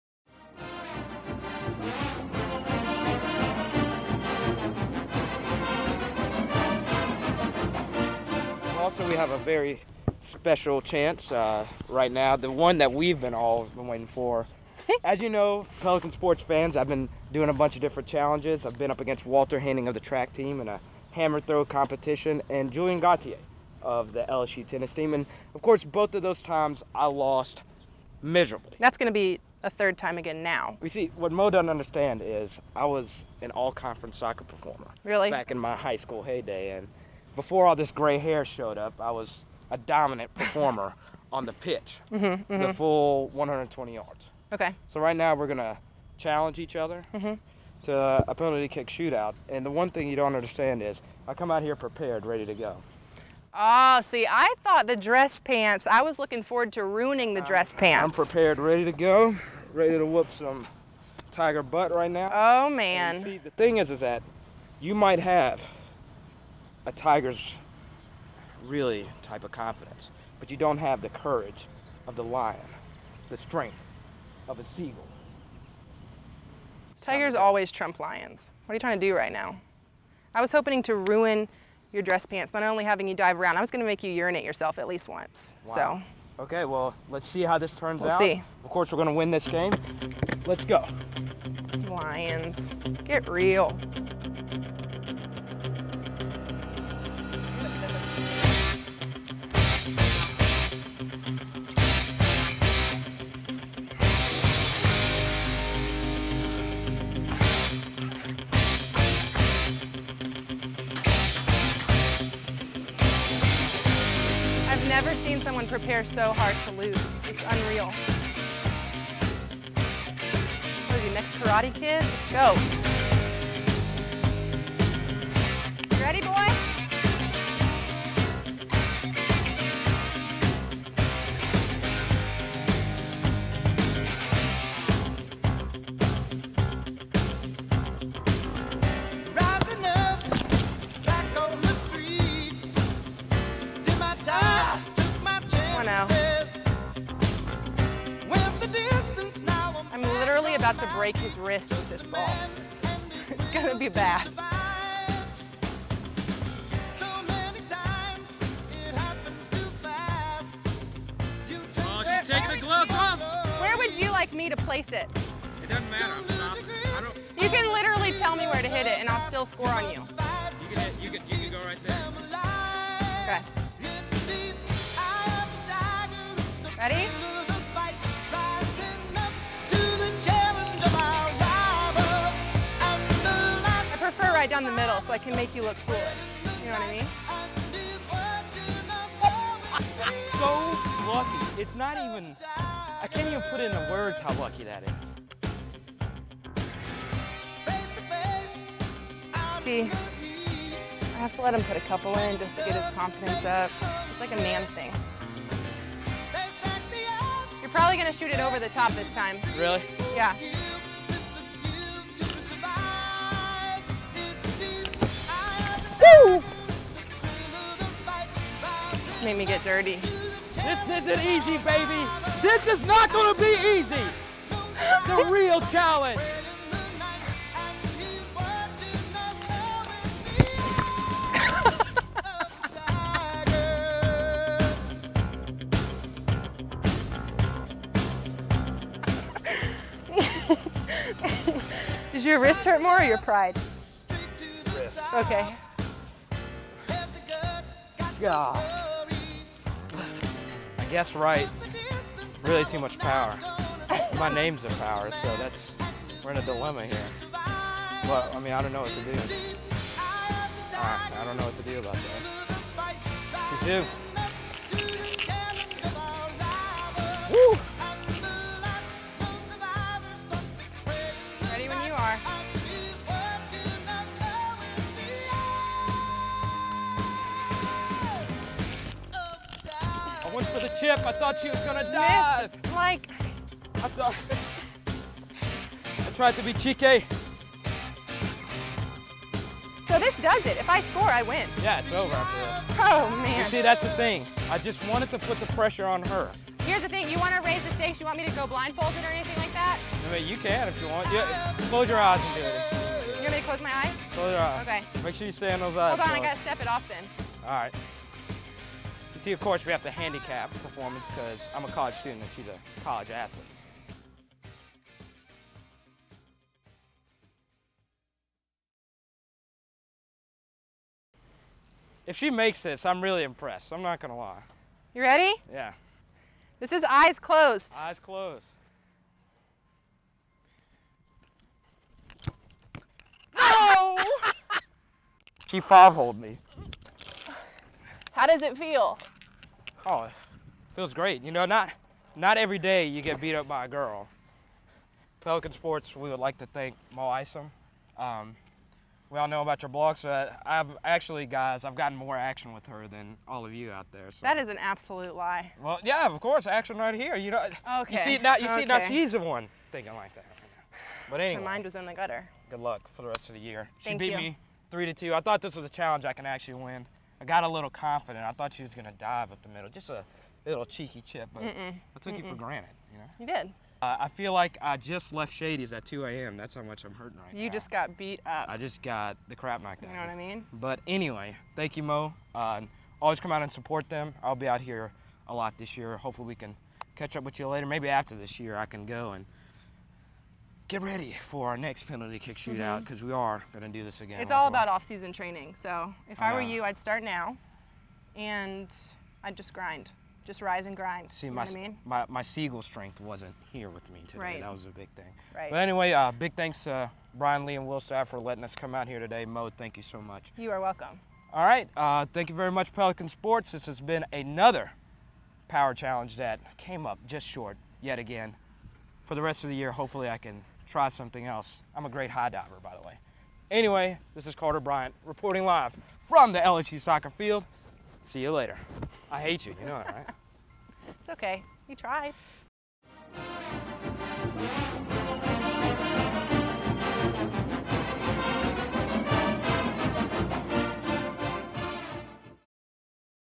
LSU Soccer (part 2) Carter Bryant vs Mo Isom - Penalty Kick Shootout
Carter Bryant challenges Mo Isom to a PK Shootout at the LSU Soccer Stadium.